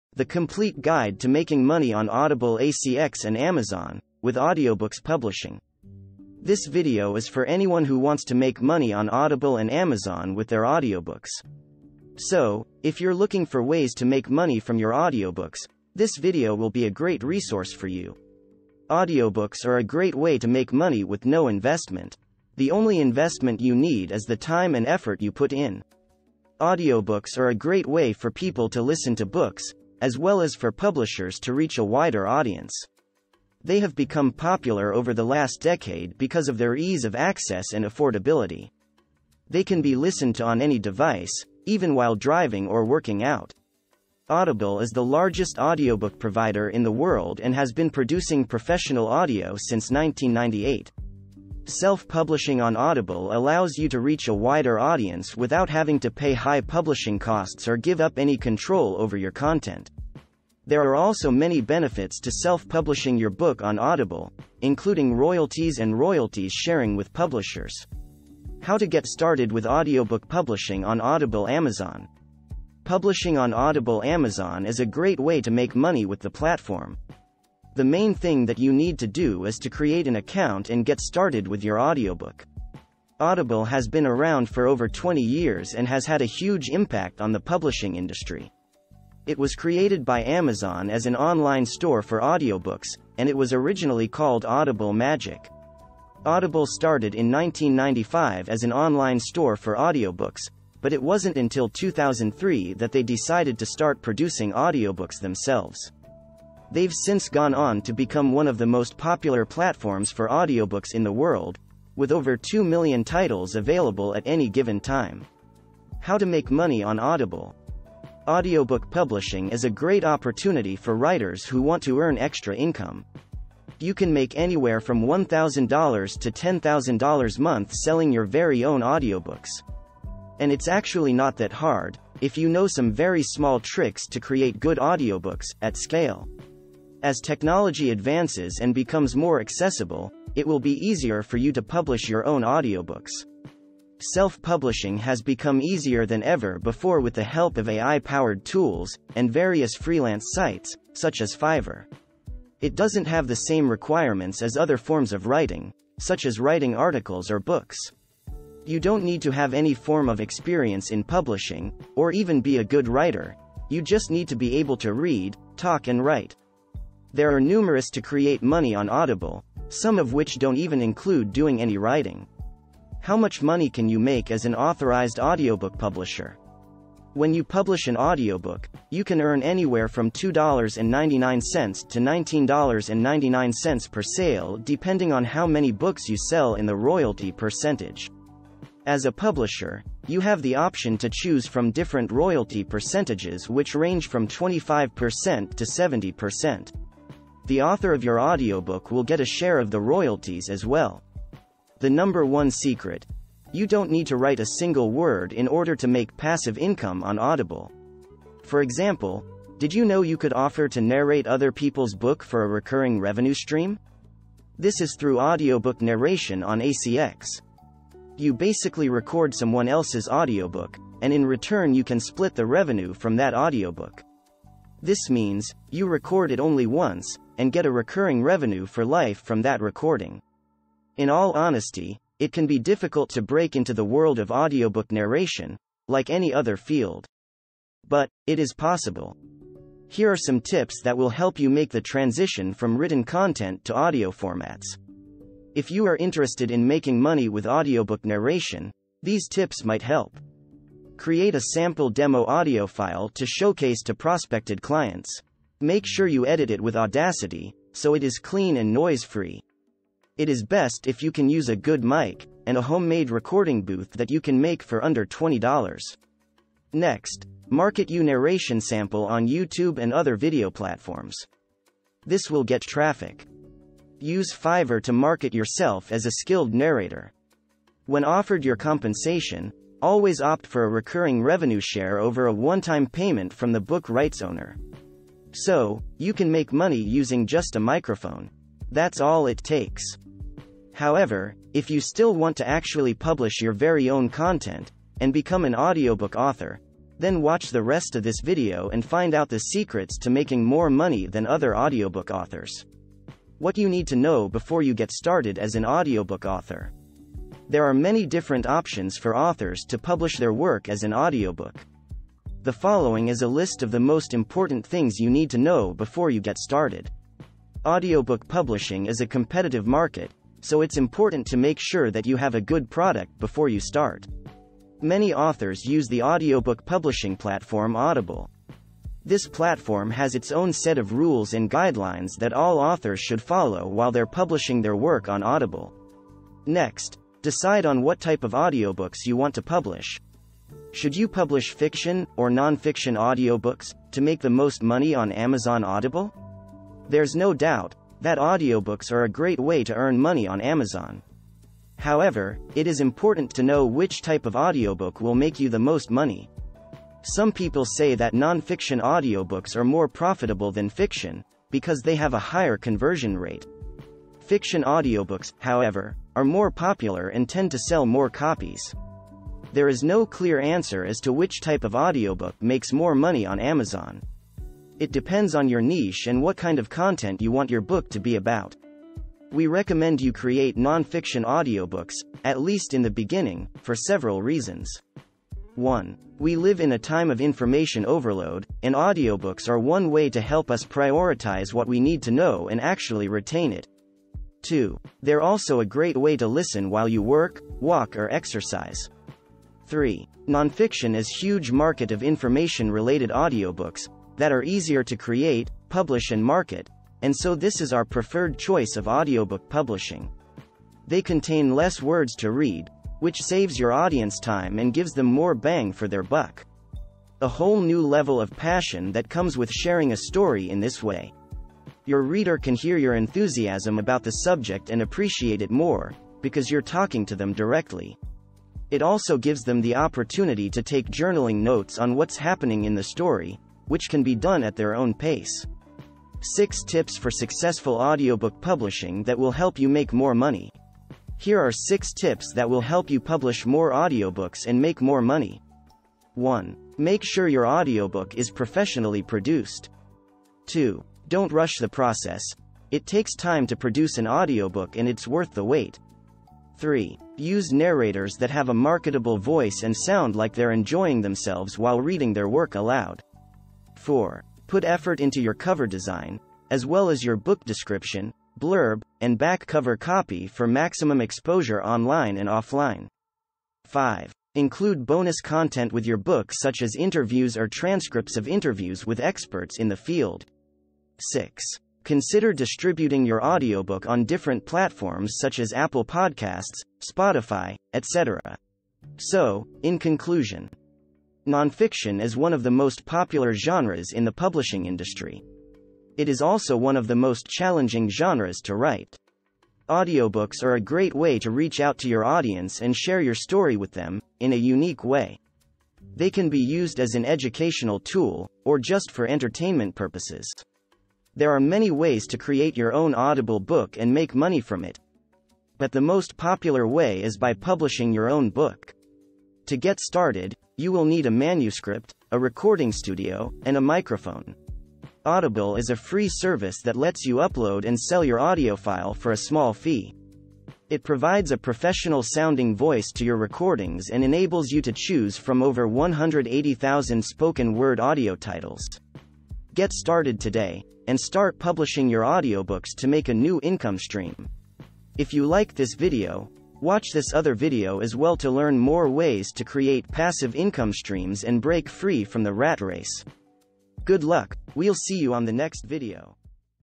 Make Money on Audible - Step by Step Guide To Making Money On Amazon ACX (2022)
Chapters:

00:00 Introduction
01:09 How to get started with publishing audiobooks on ACX
01:56 How to make money on Audible

The Complete Guide to Making Money on Audible ACX and Amazon, with Audiobooks Publishing

This video is for anyone who wants to make money on Audible and Amazon with their audiobooks.

So, If you're looking for ways to make money from your audiobooks, this video will be a great resource for you!

Audiobooks are a great way to make money with no investment. The only investment you need is the time and effort you put in.

Audiobooks are a great way for people to listen to books, as well as for publishers to reach a wider audience.

They have become popular over the last decade because of their ease of access and affordability. They can be listened to on any device, even while driving or working out.

Audible is the largest audiobook provider in the world and has been producing professional audio since 1998. Self-publishing on Audible allows you to reach a wider audience without having to pay high publishing costs or give up any control over your content.

There are also many benefits to self-publishing your book on Audible, including royalties and royalties-sharing with publishers.

How to Get Started with Audiobook Publishing on Audible Amazon

Publishing on Audible Amazon is a great way to make money with the platform. The main thing that you need to do is to create an account and get started with your audiobook.

Audible has been around for over 20 years and has had a huge impact on the publishing industry. It was created by Amazon as an online store for audio books, and it was originally called Audible Magic.

Audible started in 1995 as an online store for audio books, but it wasn't until 2003 that they decided to start producing audiobooks themselves. They've since gone on to become one of the most popular platforms for audiobooks in the world, with over 2 million titles available at any given time.

How to make money on Audible:

Audiobook publishing is a great opportunity for writers who want to earn extra income. You can make anywhere from $1,000 to $10,000 month selling your very own audiobooks!

And it’s actually not that hard, if you know some very small tricks to create good audiobooks, at scale. As technology advances and becomes more accessible, it will be easier for you to publish your own audiobooks.

Self publishing has become easier than ever before with the help of A.I powered tools, and various freelance sites, such as Fiverr.

It doesn't have the same requirements as other forms of writing, such as writing articles or books.

You don't need to have any form of experience in publishing, or even be a good writer - you just need to be able to read, talk and write!

There are numerous to create money on Audible, some of which don’t even include doing any writing.
FinanceBootcamp# FinancialFreedom